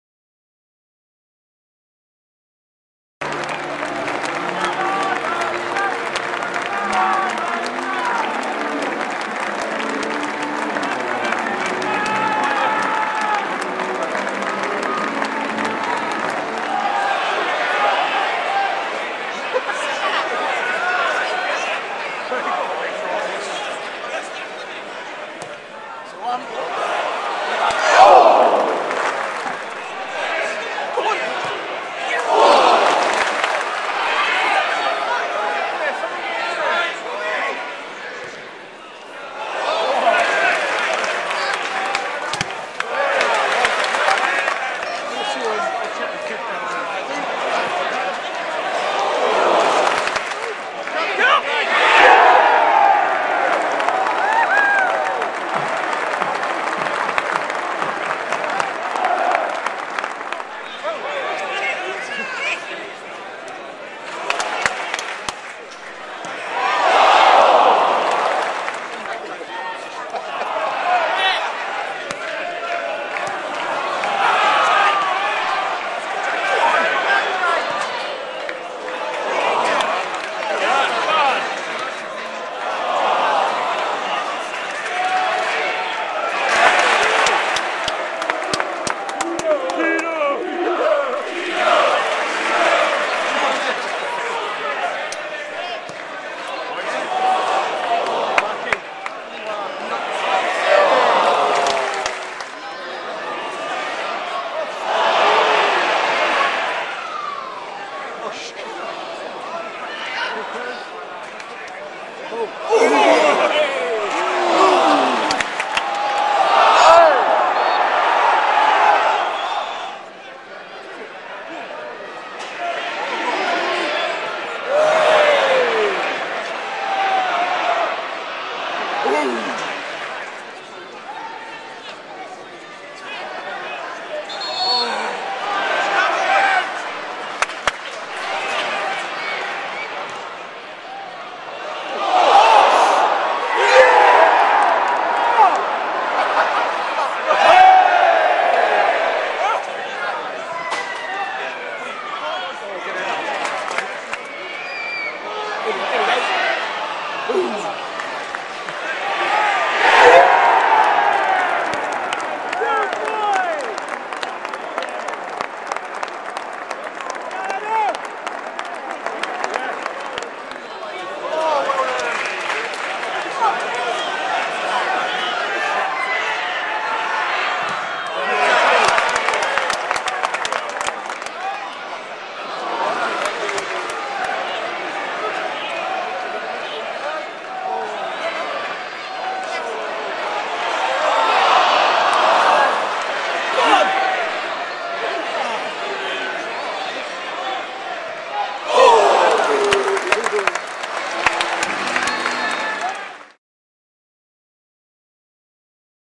Luton Town 2 Bath 0 Goals + Highlights Kovacs Watkins
Goals from Janos Kovacs and Adam Watkins helped Luton ease to victory over struggling Bath City, who have now gone eight games without a win.

Kovacs put the Hatters ahead when he found the end of Jake Howells' cross and nodded into the top corner.

Danny Crow and Alex Lawless both hit efforts straight at Romans goalkeeper Glyn Garner.

Bath's hopes of a stealing a point were ended when substitute Watkins volleyed home Lawless's deep cross.

Luton Town
01 Tyler
03 Taylor
06 Pilkington
15 Howells (Fleetwood - 83' )
16 Asafu-Adjaye
34 Kovacs Booked
04 Keane (Boucaud - 66' )
07 Lawless
11 Willmott
20 Kissock (Watkins - 55' )
10 Crow
Substitutes
17 Boucaud
28 Watkins
09 McAllister
13 Fleetwood
14 O'Connor

Bath City
01 Garner
05 Jones
15 Preece
26 Gallinagh
03 Stonehouse Booked
08 Connolly
11 Hogg (Shephard - 24' )
16 Canham
18 Watkins (Murray - 67' )
19 Burnell
27 Canham
Substitutes
04 Rollo
07 Murray
36 Shephard
40 Doherty
09 Cook

Ref: Whitton
Att: 5,745 .